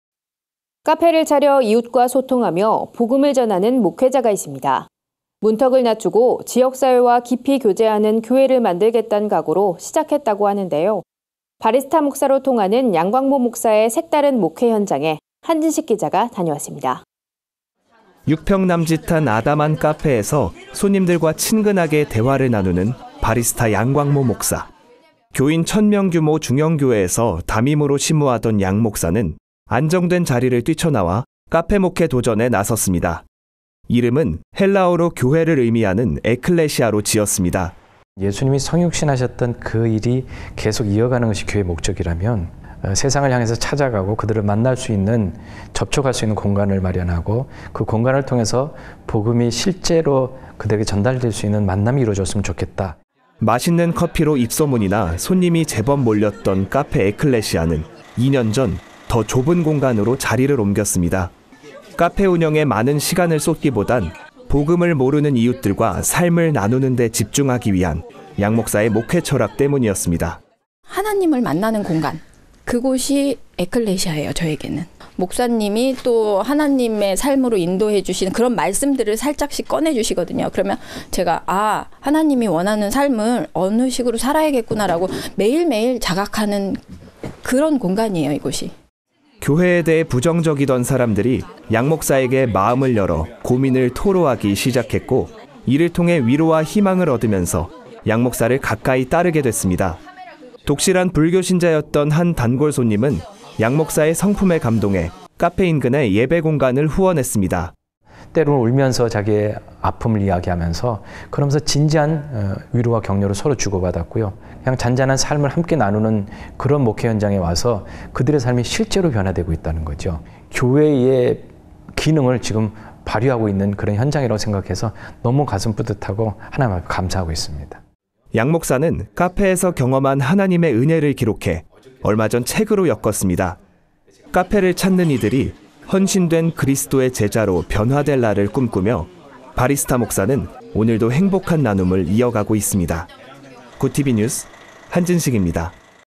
[GOODTV NEWS 20180129] 커피로 복음 전하는 바리스타 목사
카페를 차려 이웃과 소통하며 복음을 전하는 목회자가 있습니다. 문턱을 낮추고 지역 주민들과 교제하는 교회를 만들겠단 각오로 시작했다고 하는데요. 바리스타 목사로 통하는 양광모 목사의 색다른 목회 현장에 한진식 기자가 다녀왔습니다.

6평 남짓한 아담한 카페에서 손님들과 친근하게 대화를 나누는  바리스타 양광모 목사.

교인 천 명 규모 중형교회에서 담임으로 시무하던 양 목사는 안정된 자리를 뛰쳐나와 카페 목회 도전에 나섰습니다. 이름은 헬라어로 교회를 의미하는 '에클레시아'로 지었습니다.

(인터뷰: 양광모 목사 / 카페 에클레시아 바리스타)

맛있는 커피로 입소문이 나 손님이 제법 몰렸던 카페 에클레시아는 2년 전 더 좁은 공간으로 자리를 옮겼습니다.

카페 운영에 많은 시간을 쏟기 보단 복음을 모르는 이웃들과 삶을 나누는 데 집중하기 위한 양 목사의 목회철학 때문이었습니다.

(인터뷰: 배은정(40) / 카페 단골고객)

교회에 대해 부정적이던 사람들이 양 목사에게 마음을 열어 고민을 토로하기 시작했고, 이를 통해 위로와 희망을 얻으면서 양 목사를 가까이 따르게 됐습니다.

독실한 불교신자였던 한 단골손님은은 양 목사의 성품에 감동해 카페 인근에 예배 공간을 후원했습니다.

양 목사는 카페에서 경험한 하나님의 은혜를 기록해 얼마 전 책으로 엮었습니다.

카페를 찾는 이들이 헌신된 그리스도의 제자로 변화될 날을 꿈꾸며 그는 오늘도 행복한 나눔을 이어가고 있습니다.

GOODTV NEWS 한진식입니다.